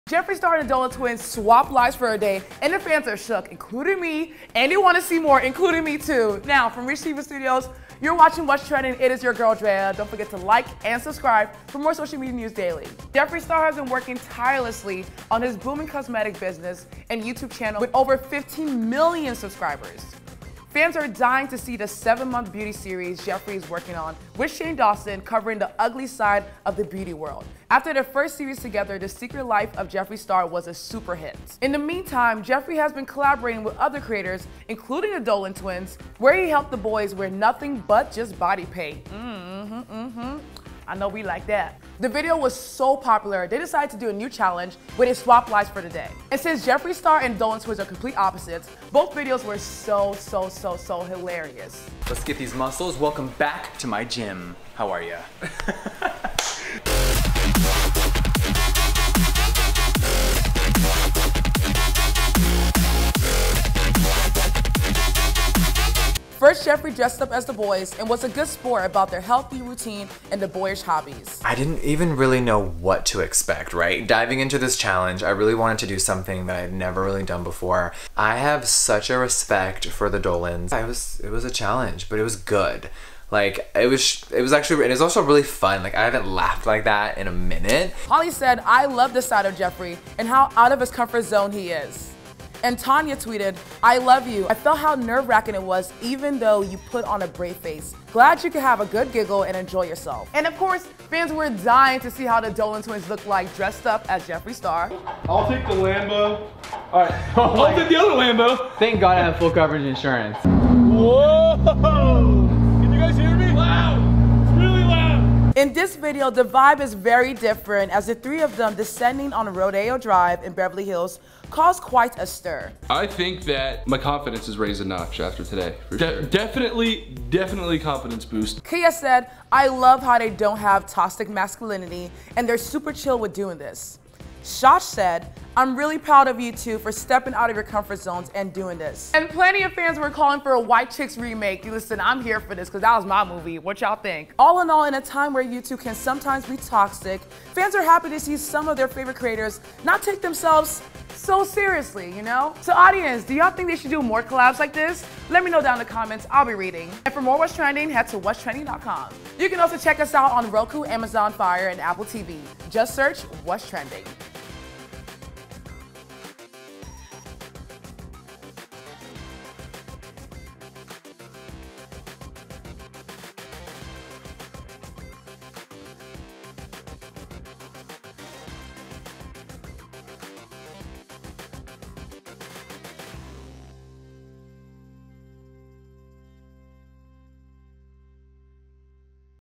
Fans SHOOK By Jeffree Star's Dolan Twins Makeover
Jeffree Star and The Dolan Twins have fans shook after they decided to switch lives for a day, and fans are dying to see more.